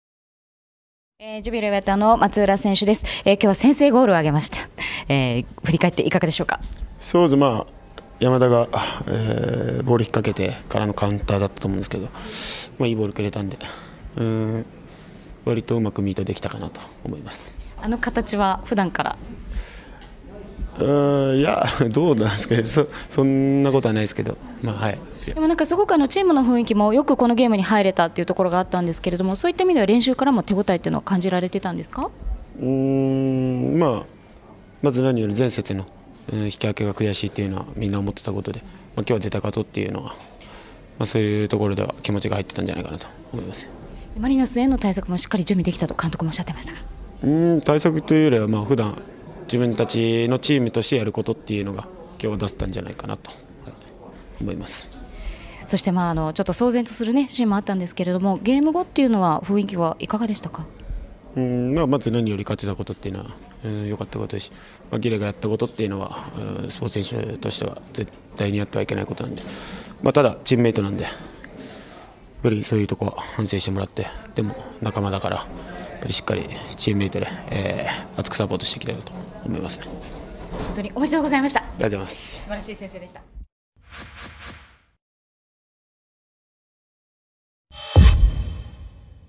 【公式】試合後インタビュー動画：松浦 拓弥（磐田）横浜Ｆ・マリノスvsジュビロ磐田 明治安田生命Ｊ１リーグ 第12節 2018/5/2
2018年5月2日（水）に行われた明治安田生命Ｊ１リーグ 第12節 横浜FMvs磐田の試合後に収録された松浦　拓弥（磐田）のインタビュー動画です。